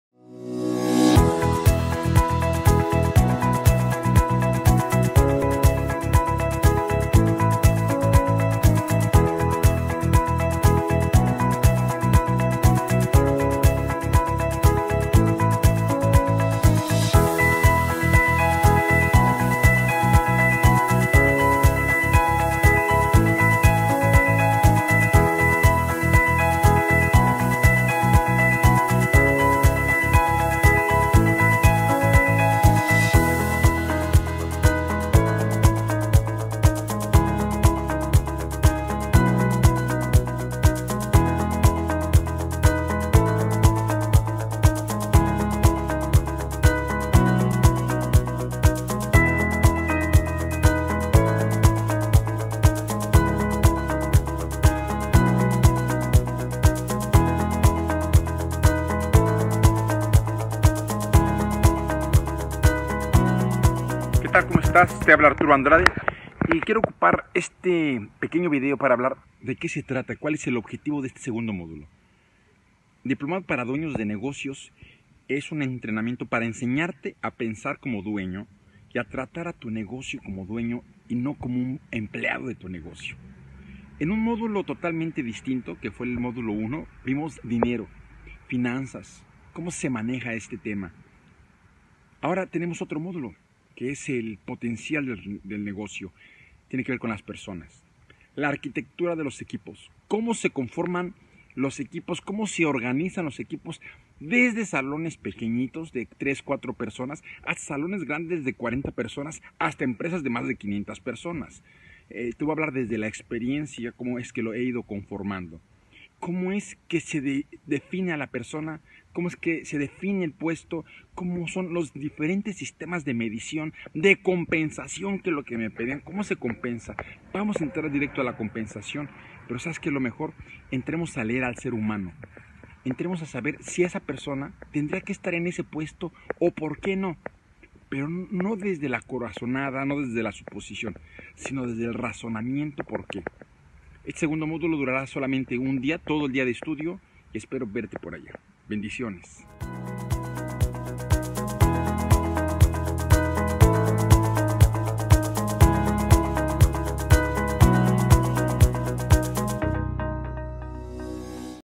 DDN DIPLOMADO PARA DUEÑOS DE NEGOCIOS (SEGUNDO MODULO)
EL COACH "ARTURO ANDRADE" NOS DÁ UNA BREVE DESCRIPCION DE LO QUE SERÁ SU NUEVO DIPLOMADO DE 2018